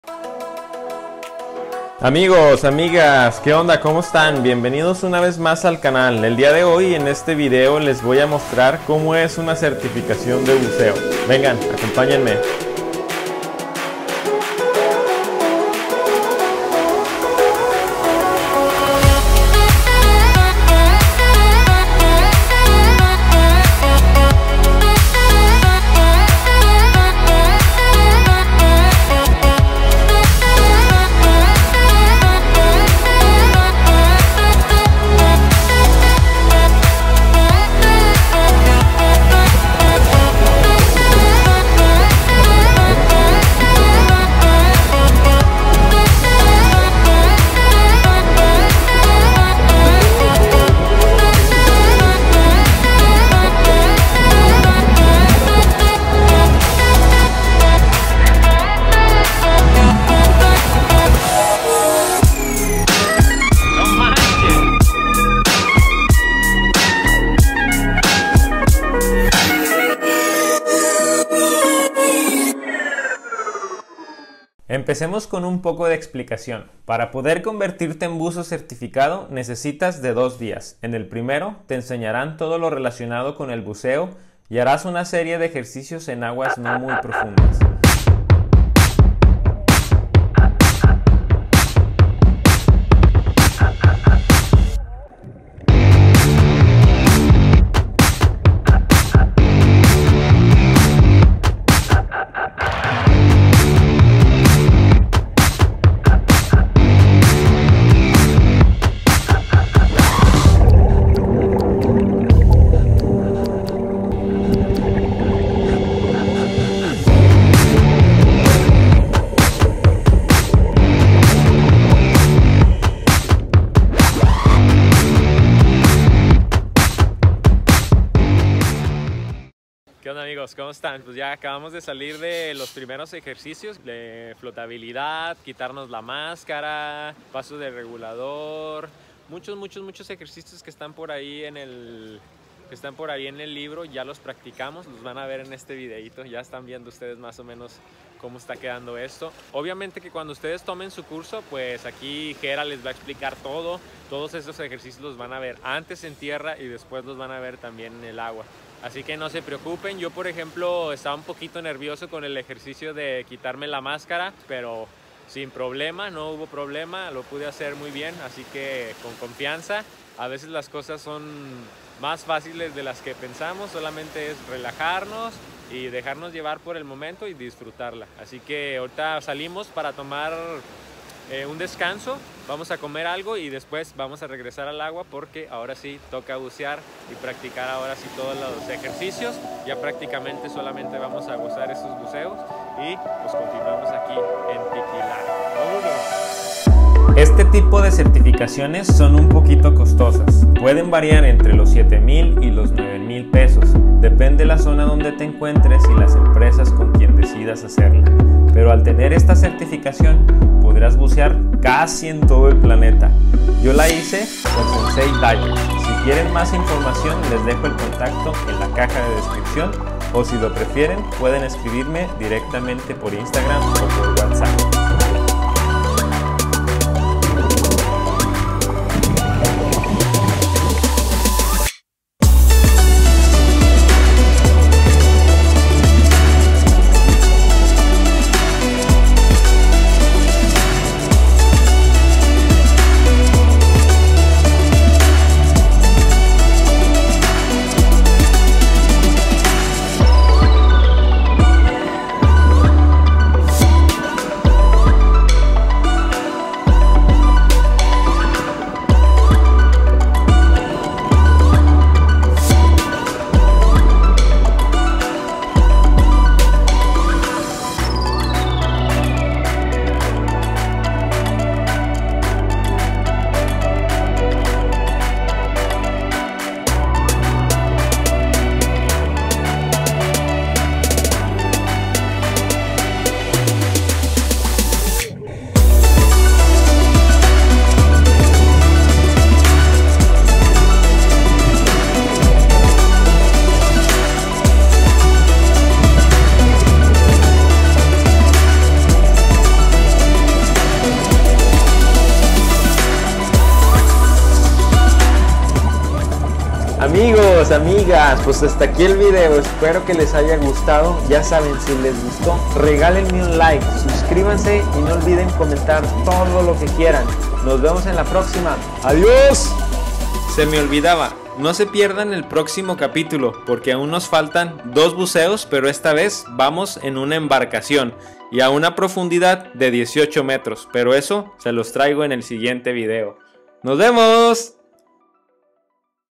BUCEO EN COZUMEL / CERTIFICACION OPEN WATER / Qué se necesita para ser buzo ? JOSS BARRON
Finalmente tome mi certificación de buceo en aguas abiertas en la maravillosa isla de Cozumel.

Con este tipo de certificación podrás bucear en casi todo el planeta y puedes bajar a una profundidad máxima de 18m.

Nuestra Certificación la hicimos con @senseidivers.

Si les gustaria apoyarme para poder mejorar el contenido de este canal les dejo mi enlace de paypal para donaciones, muchisimas gracias.

POR SI TIENEN ALGUNA DUDA O ME QUIEREN SALUDAR LES DEJO MIS OTRAS REDES: